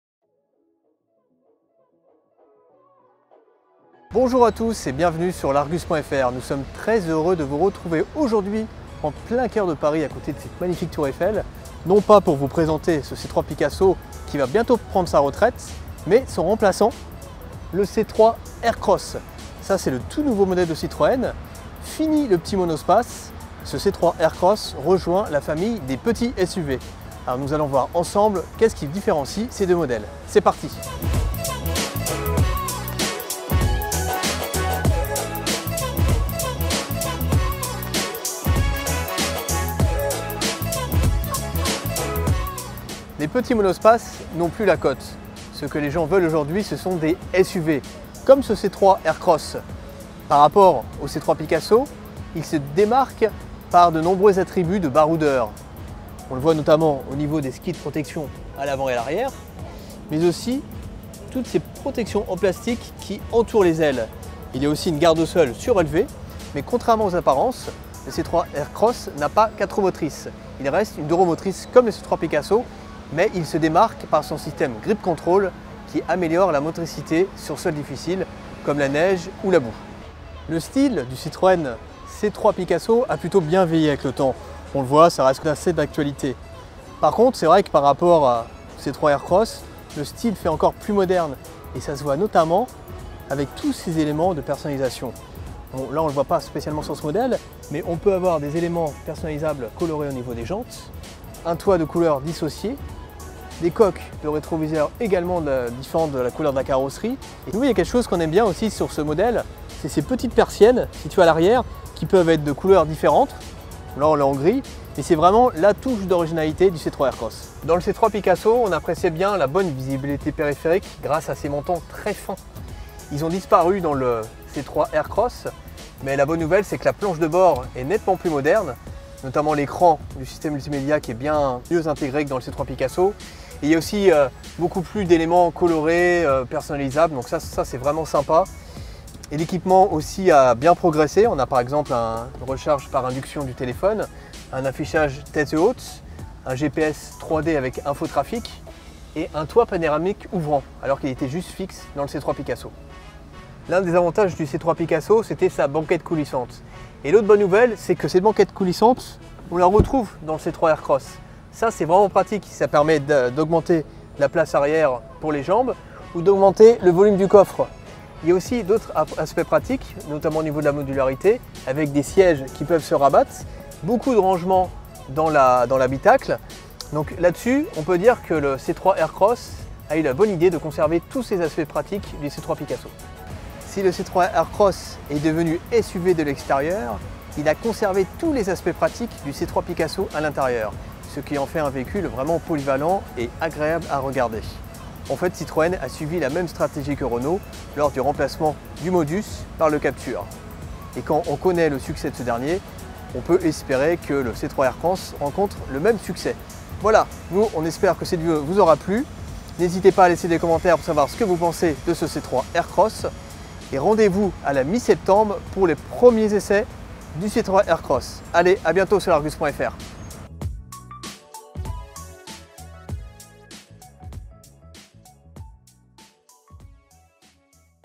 Citroën C3 Aircross vs C3 Picasso : quelles sont les différences ?
Le Citroën C3 Aircross est un petit SUV qui va prendre la relève du C3 Picasso à l'automne 2017. Design, équipement, modularité, quels sont les grands changements ? Découverte.